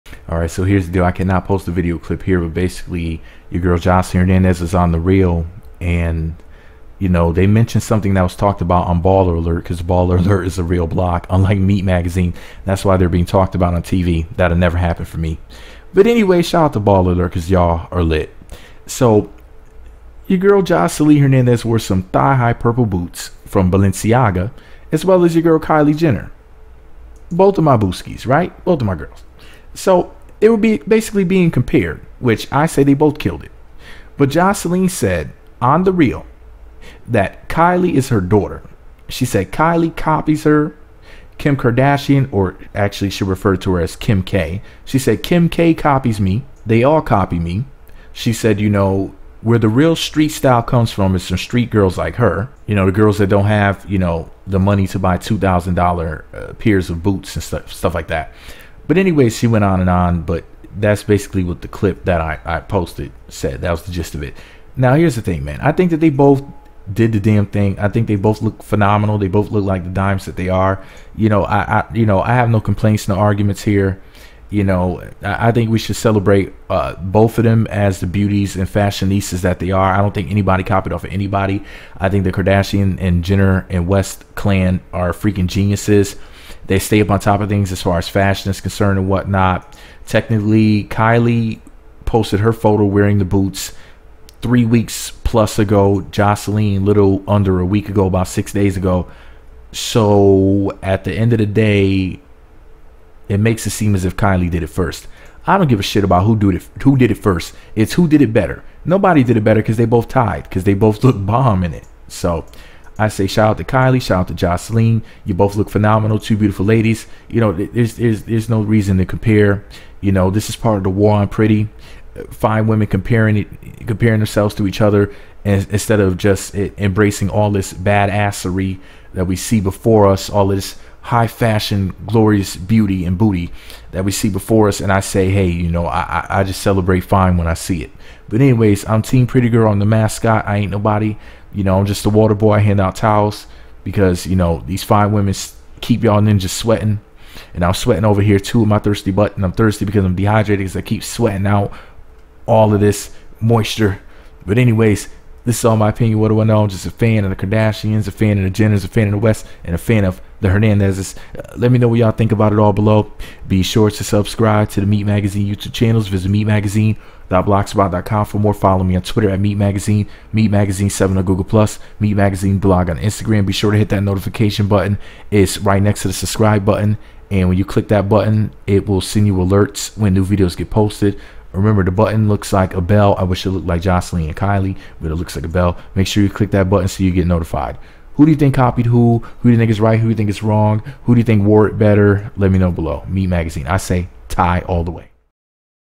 Joseline fight w/ Kim Kardashian & Kylie Jenner over purple boots & fashion starts here!
Joseline Hernandez fight with Kim Kardashian & Kylie Jenner over purple Balenciaga boots & fashion starts here! HOT Love and Hip Hop Atlanta Season 6 star says the K Girls get their style from her and Urban black women like her that grew up without money! LMAO! Too many pretty girls for me to pick a side!